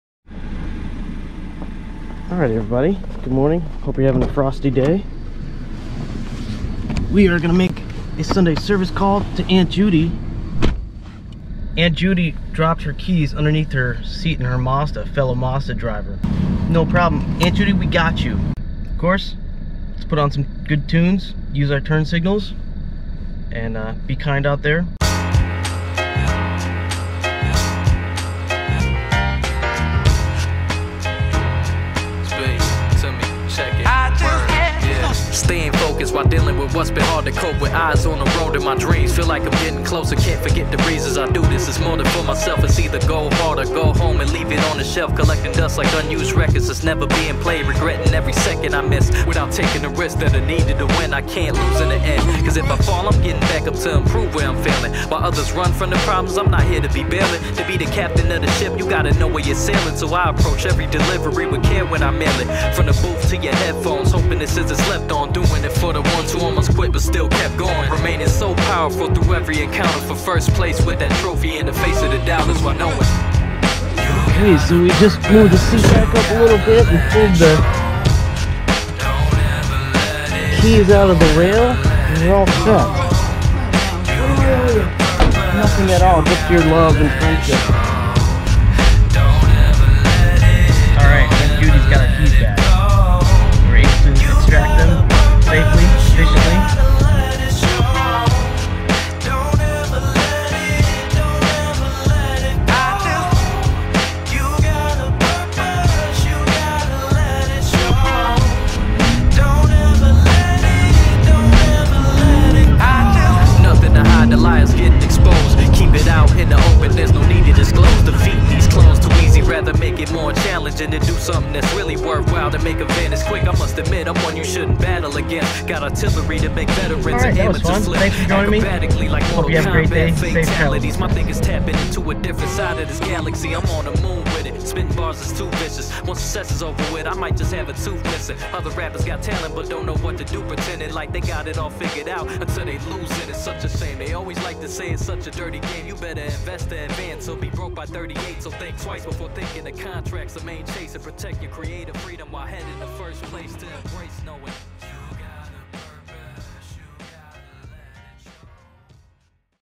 992 Cast Ep 55 "Aunt Judy"
A service call came in for "Keys stuck underneath the seat" with Aunt Judy a fellow Mazda 3 driver!